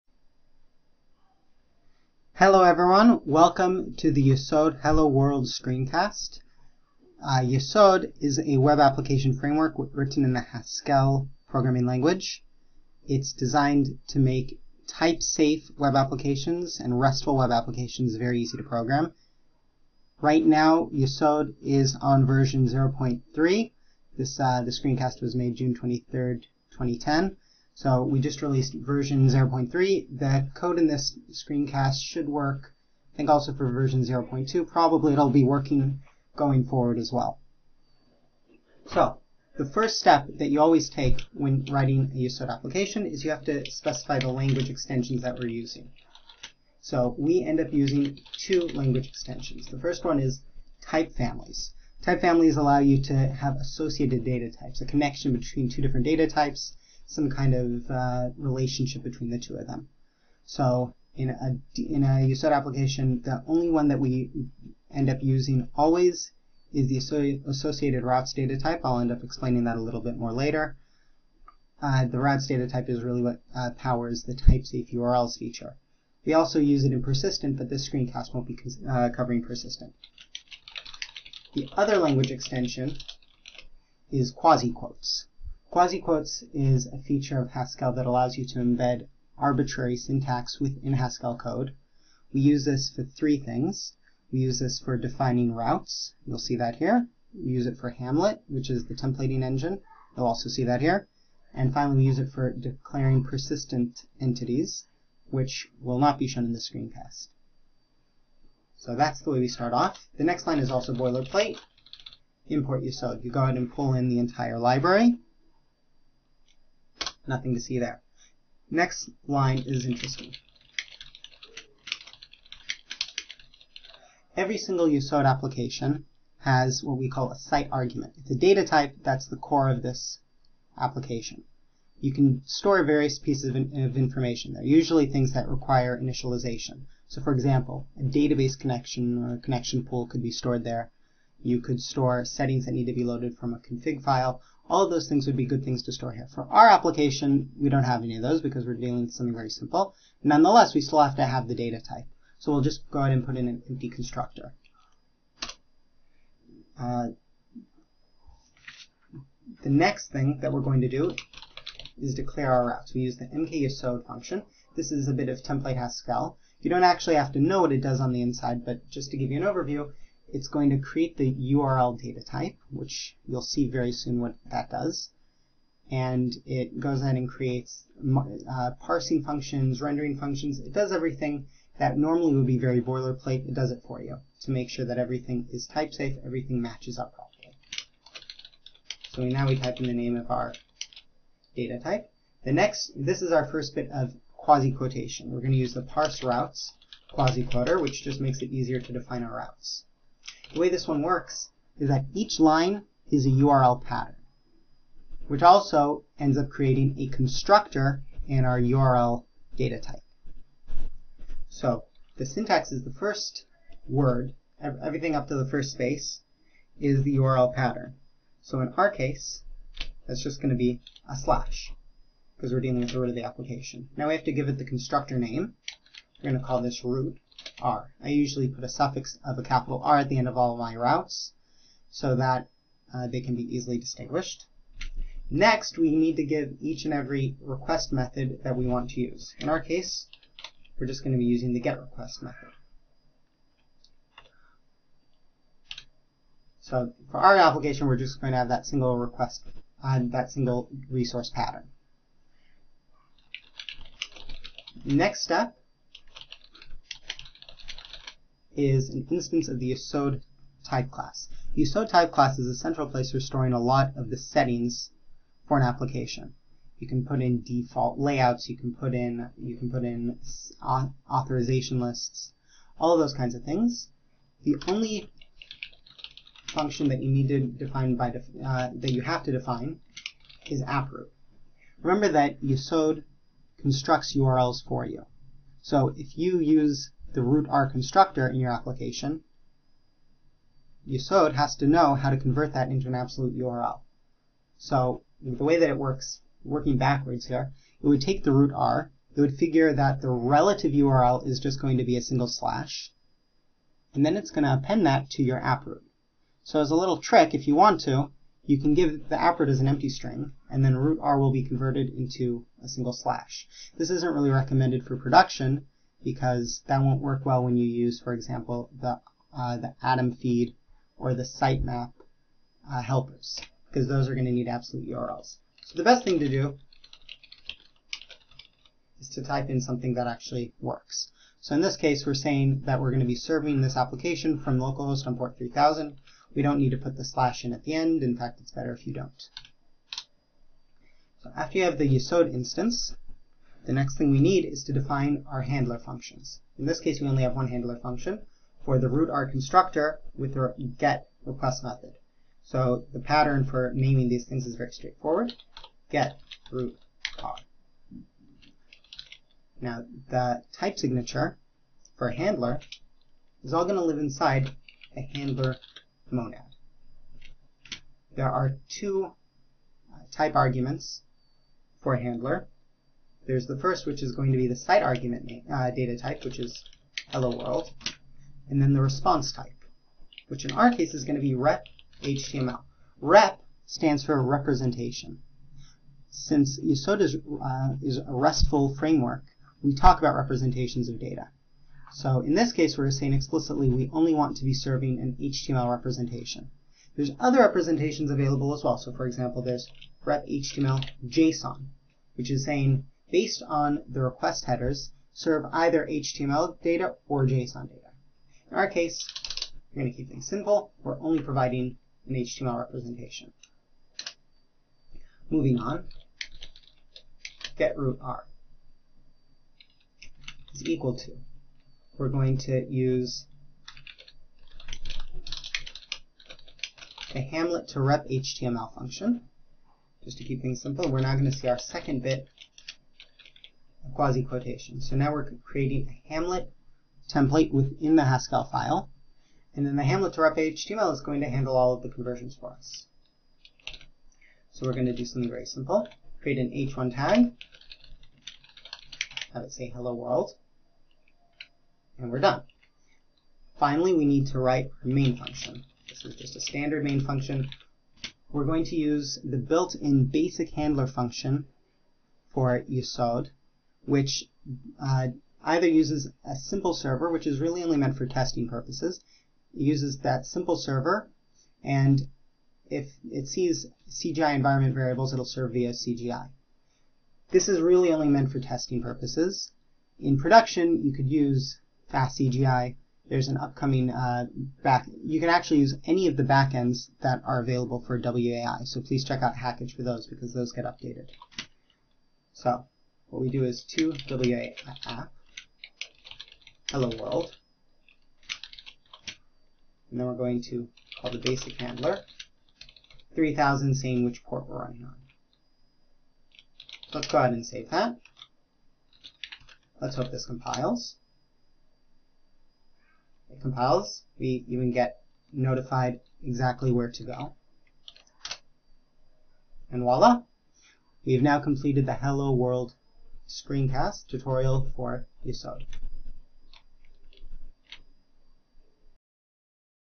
Yesod Web Framework - Hello World
A hello world screencast, walking through some of the basics of the Yesod Web Framework. Should give a good introduction to newcomers.